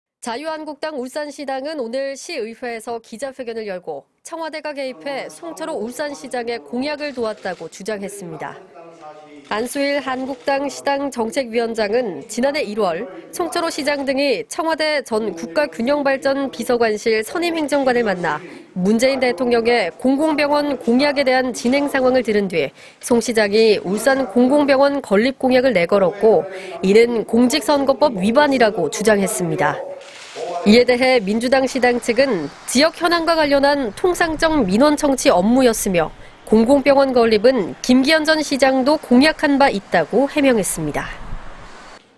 191211 한국당 울산시당, "청와대가송철호 울산시장 공약 만들어줘" [ubc울산방송]
자유한국당 울산시당은

오늘(11) 시의회에서

기자회견을 열고 "청와대가

개입해 송철호 울산시장의

공약을 도왔다"고 주장했습니다.

 안수일 한국당 시당

정책위원장은 "지난해 1월

송철호 시장 등이 청와대 전

국가균형발전 비서관실

선임행정관을 만나 문재인

대통령의 공공병원 공약에 대한 진행 상황을 들은 뒤 송 시장이 울산 공공병원 건립 공약을

내걸었고 이는 공직선거법

위반"이라고 주장했습니다.

 이에 대해 민주당 시당 측은

"지역 현안과 관련한 통상적

민원청취 업무였으며 공공병원 건립은 김기현 전 시장도 공약한 바 있다"고 해명했습니다.